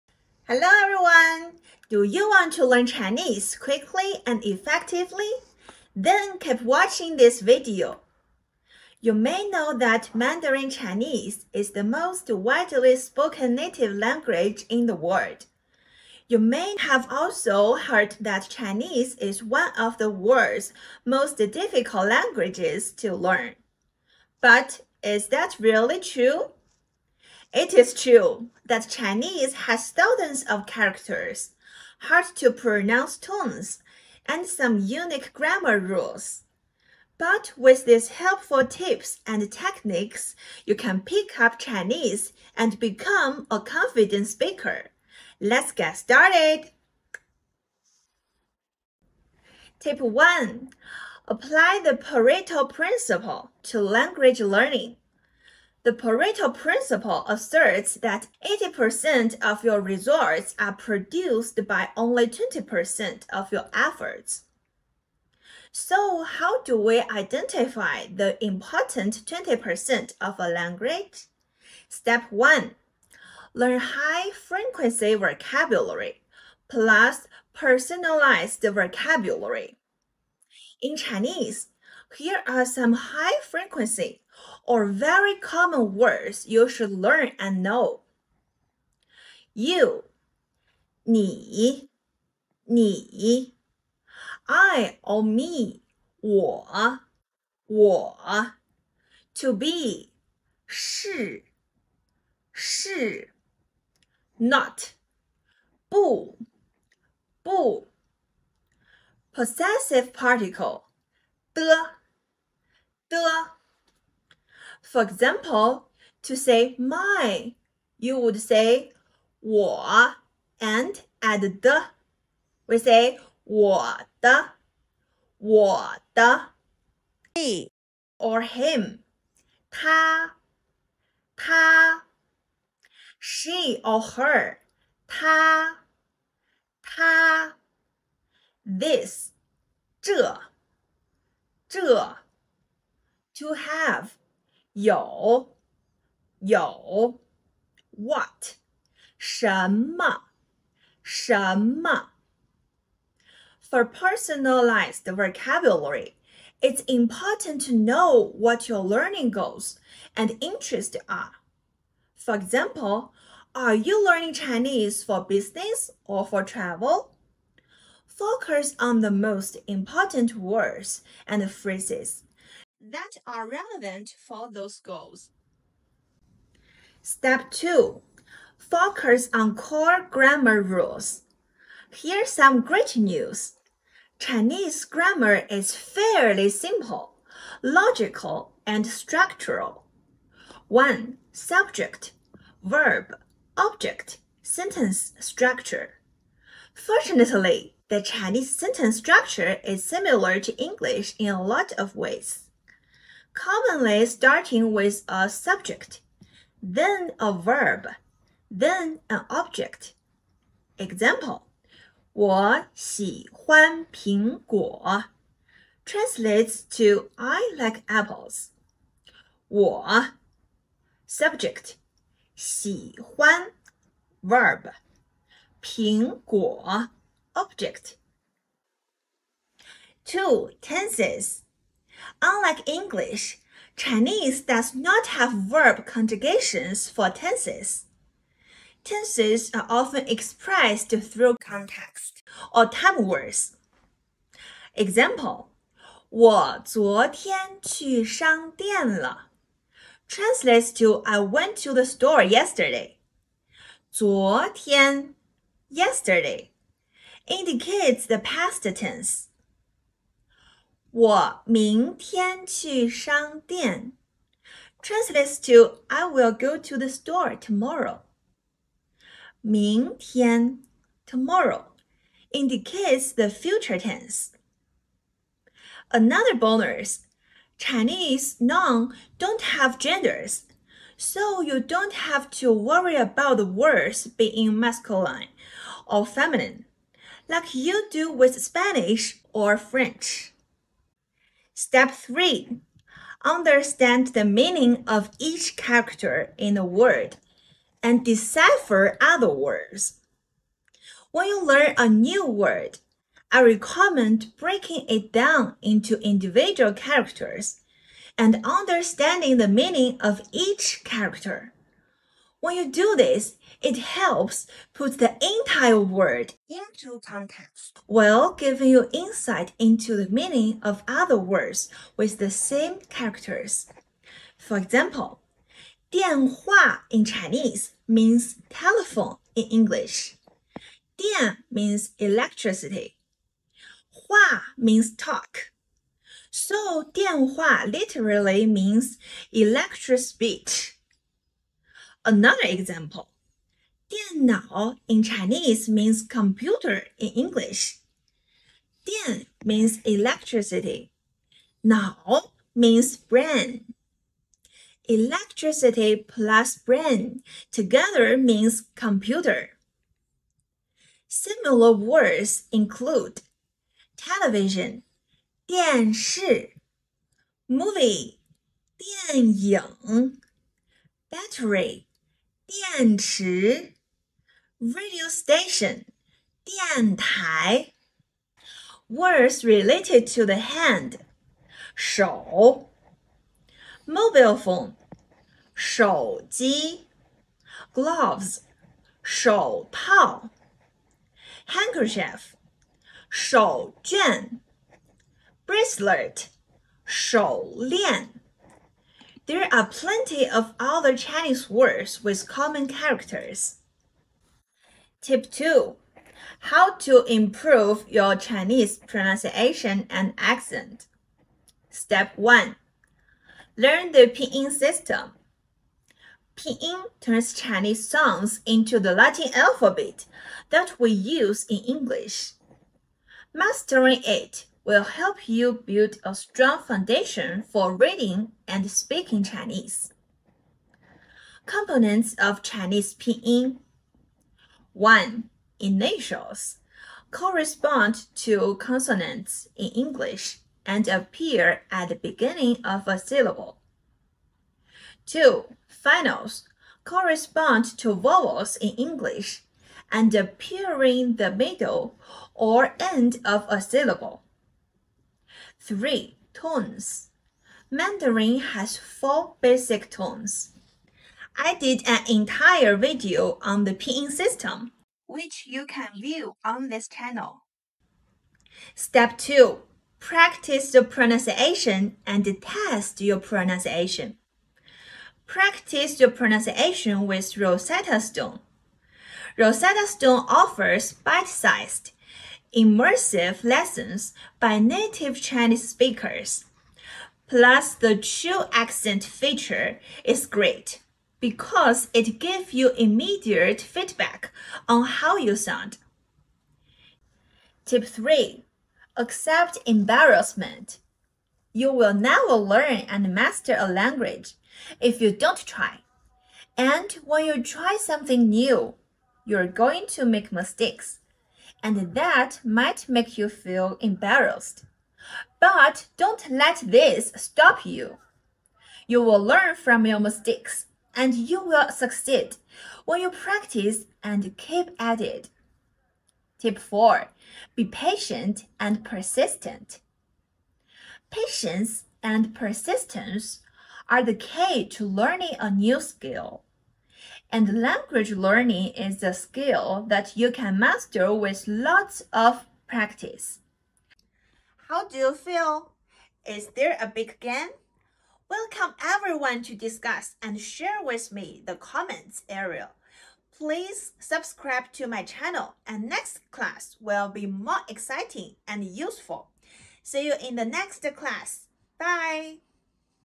Learn Chinese quickly and effectively(Chinese learning skills）！！！
Hello my dear friends, I am Chinese teacher beauty.
Chinese is easy.
If you want to learn Chinese easily, quickly and effectively;
If you want to master Chinese pronunciation rules and common Chinese rules;
If you want to learn authentic Chinese and use it to communicate naturally.

Chinese has thousands of characters, hard to pronounce tones, and some unique grammar rules, but with these helpful tips and techniques, you can pick up Chinese and become a confident speaker.

00:00 Intro
00:48 Tip 1: Apply the Pareto Principle (also known as the 80/20 rule) to language learning
06:44 Tip 2: How to improve your Chinese pronunciation and accent
08:17 Tip 3: Accept embarrassment
08:45 Tip 4: Be patient and persistent